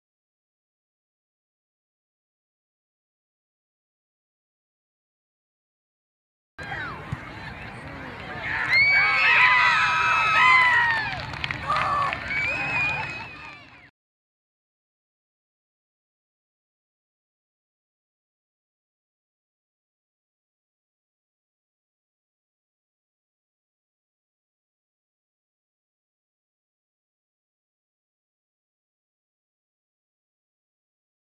Aimee Goal 2 - Rockets Game 3 - Storm Classic Tournament
Rockets Game 3 of the Storm Tournament in Centennial Colorado - June 2009.